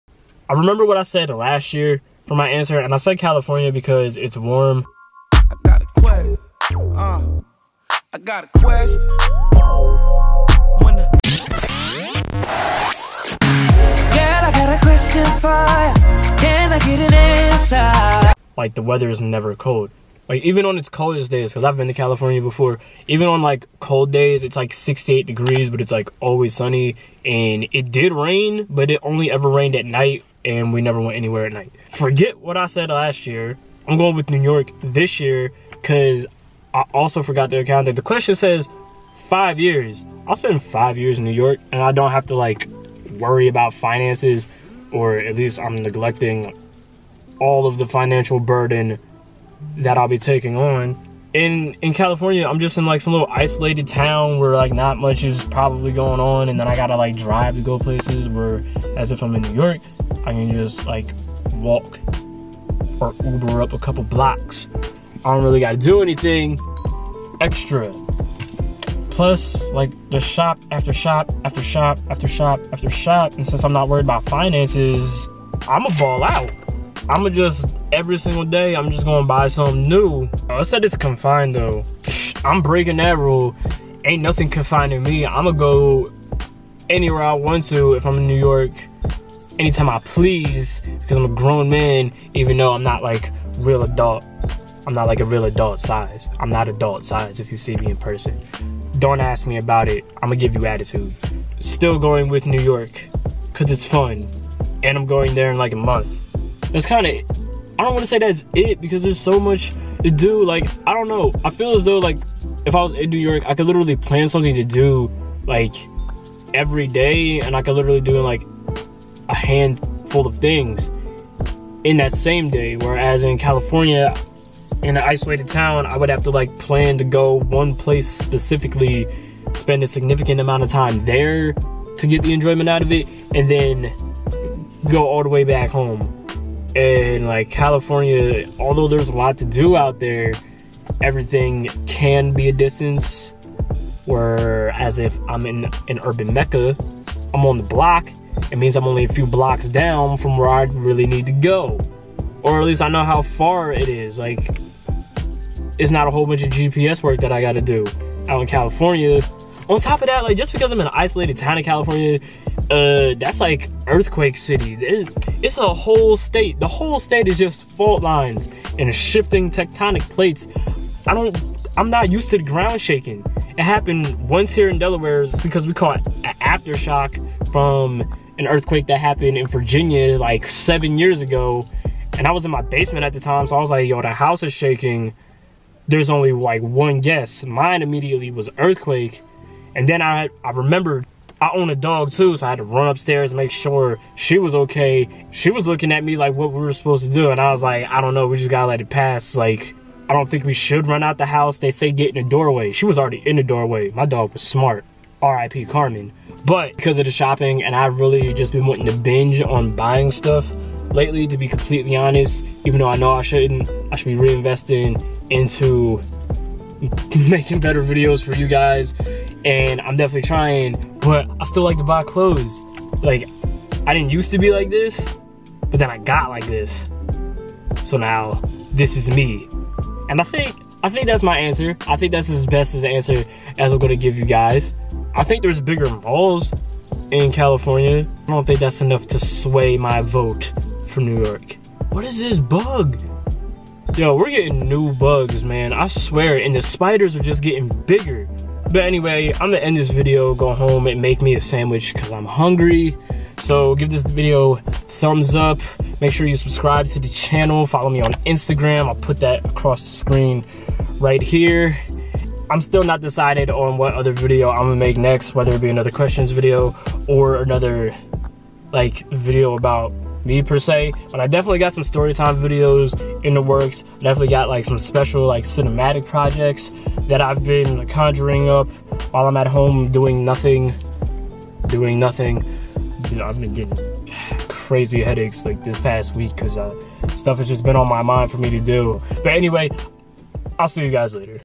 Question 4: New York or California?🌃🌉
Ignoring all financial considerations, would you rather spend the next 5 years confined to an urban mecca like New York, or  beautiful, isolated town on the California coast? I side with living in New York this time, rather than California, I think I could take those harsh winters for 5 years.